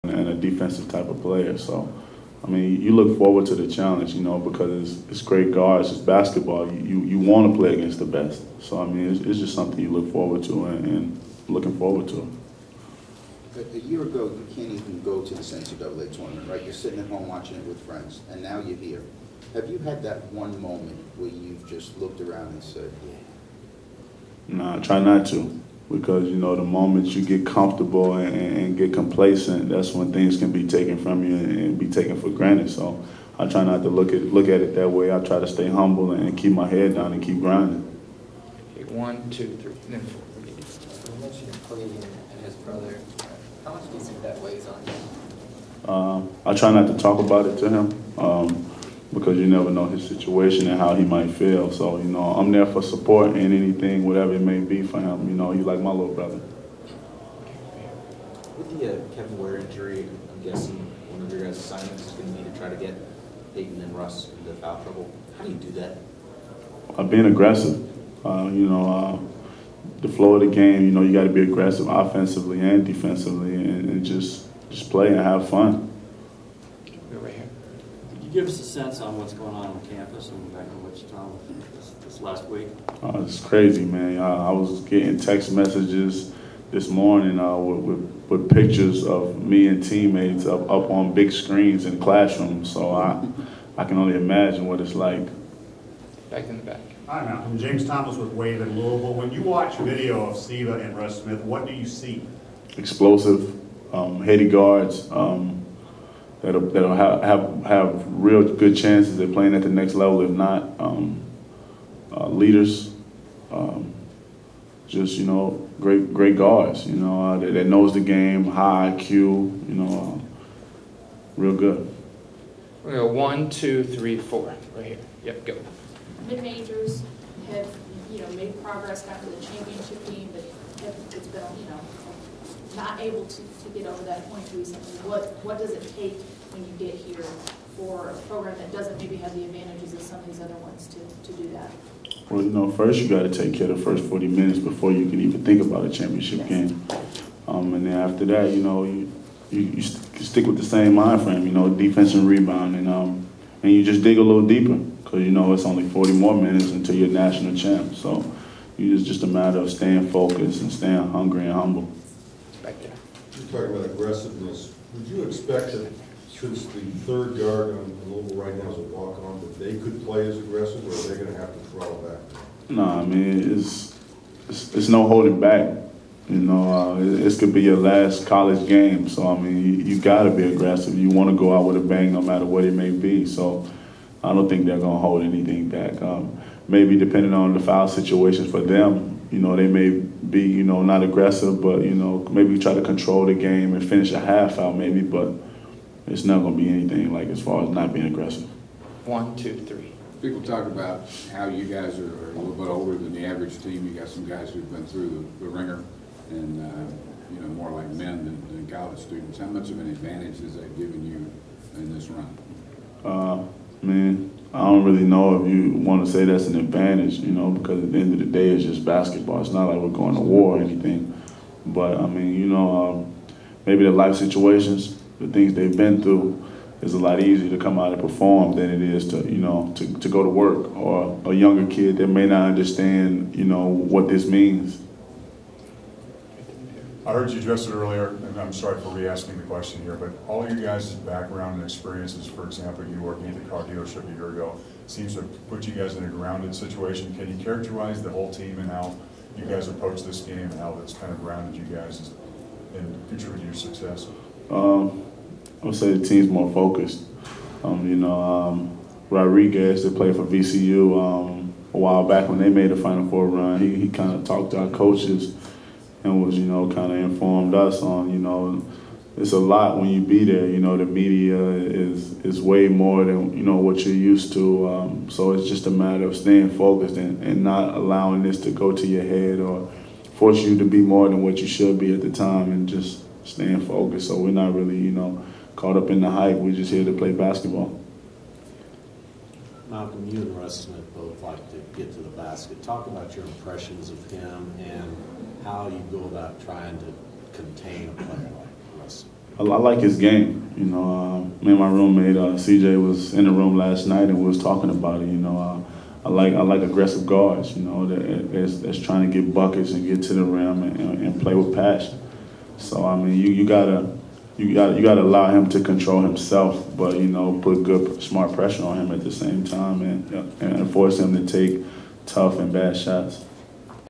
Wichita State's Malcolm Armstead Talks Final Four vs. Louisville 4-5-2013
Wichita State Shocker Malcolm Armstead Talks Final Four vs. Louisville.  Wichita State will face the Louisville Cardinals Saturday at Atlanta's Georgia Dome in the National Semifinals in the NCAA's Final Four.